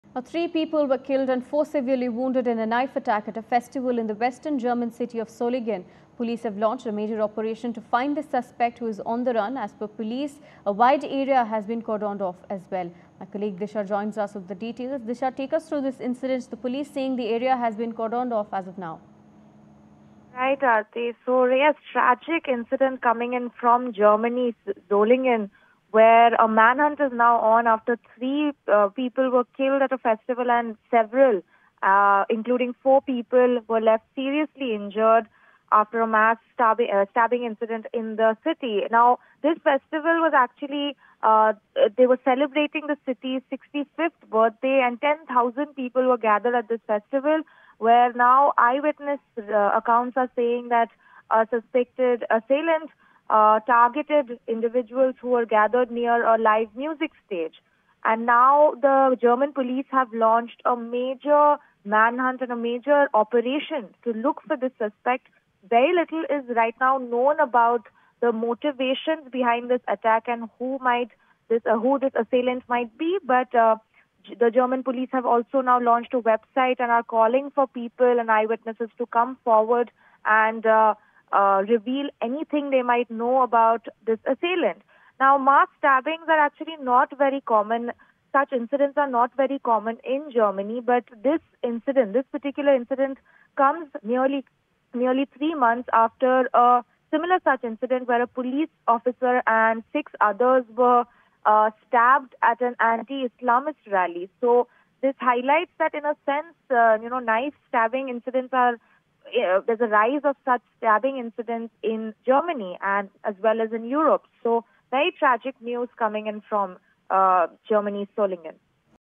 Germany Stabbing | Police Search For Assailant After 3 Killed In Germany Stabbing Incident
German police said on Saturday that they were still searching for an unknown assailant hours after he killed three people and wounded others in a stabbing attack at a festival in the western city of Solingen. Eight were injured, five of them seriously, the police said in a statement early on Saturday. That is up from the four serious injuries the police reported earlier.